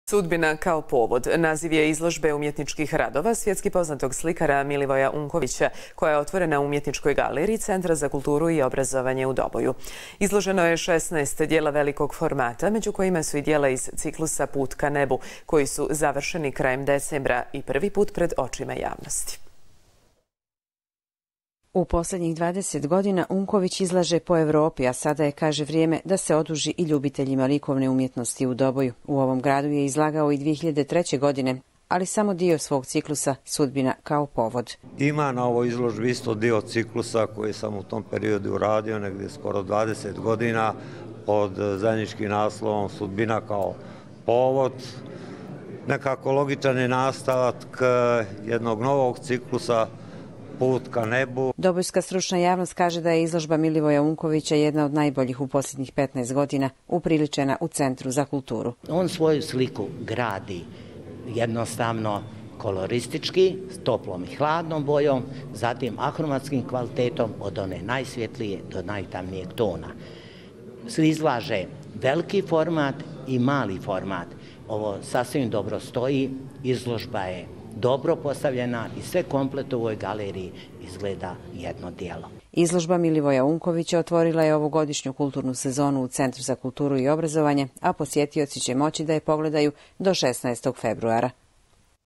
Izložba Milivoja Unkovića u Doboju
"Sudbina kao povod", naziv je izložbe umjetničkih radova svjetski poznatog slikara Milivoja Unkovića koja je otvorena u Umjetničkoj galeriji Centra za kulturu i obrazovanje u Doboju.